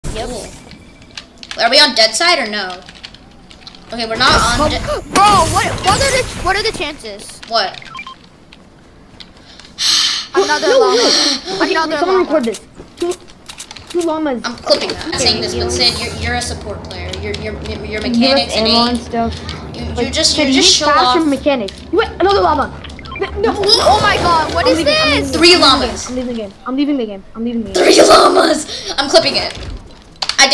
3 llamas in 3 arena games
Im going to switch to arena highlights instead of montages because I don't really hit clips anymore and no one really likes them anymore